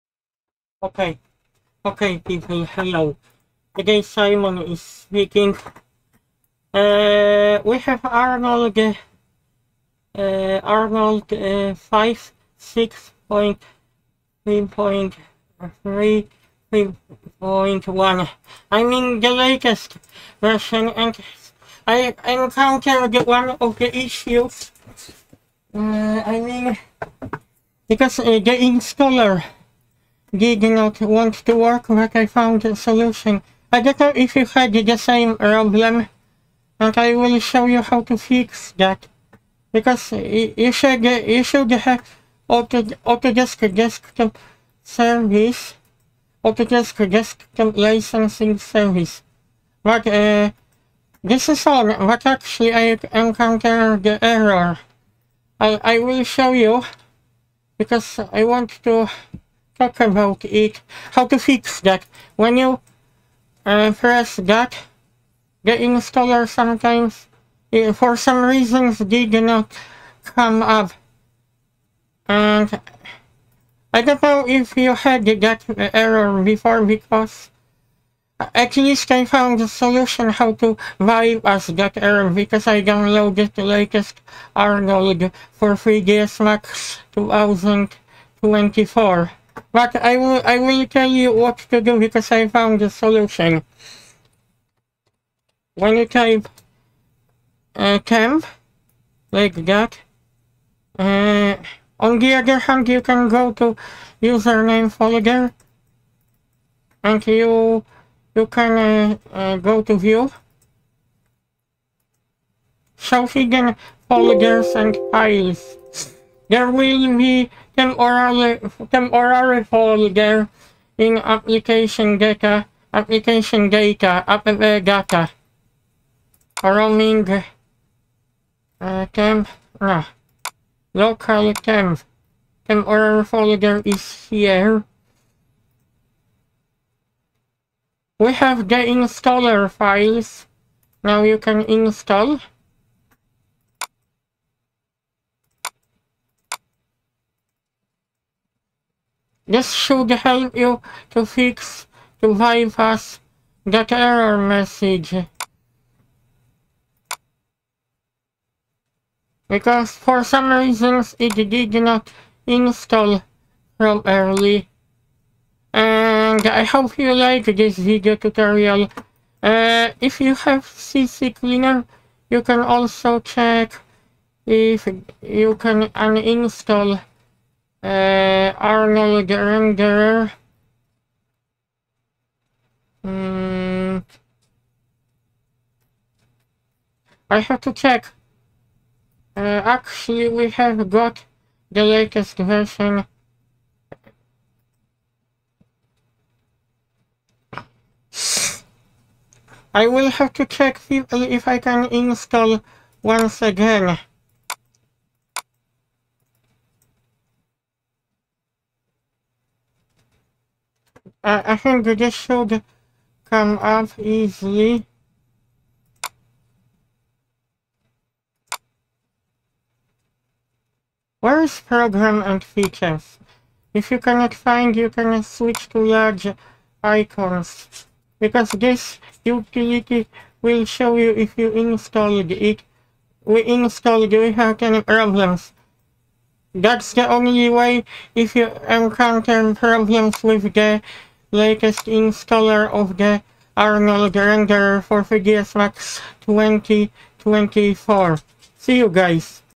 3ds max 2024 - how to fix - Arnold renderer (5.6.3.1) - will not install - tutorial EN 2023
3ds max 2024 - how to fix - Arnold renderer (5.6.3.1) - will not install - tutorial EN 2023